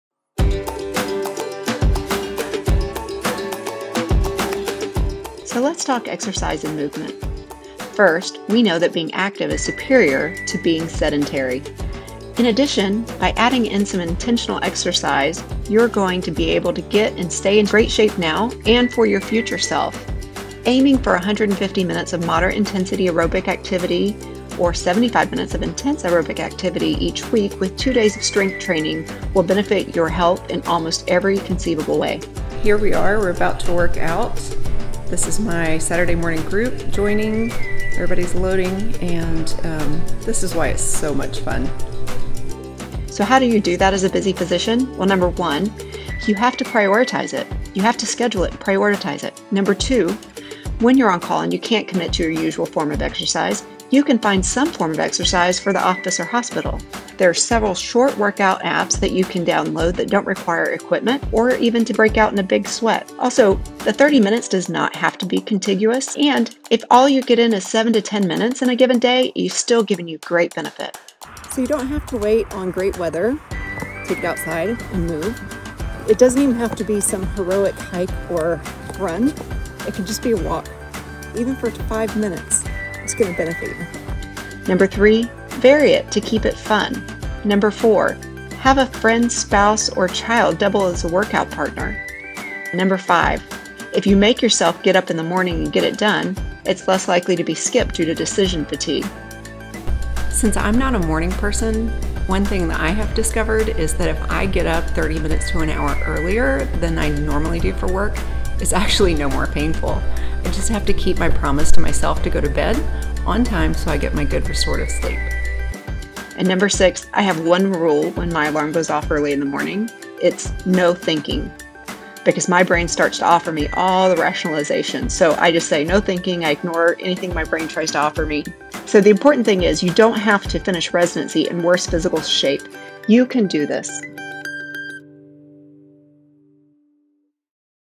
Exercise?! As a busy family physician?
In the October 2020 vlog, I offer some encouragement about moving for family medicine residents and faculty.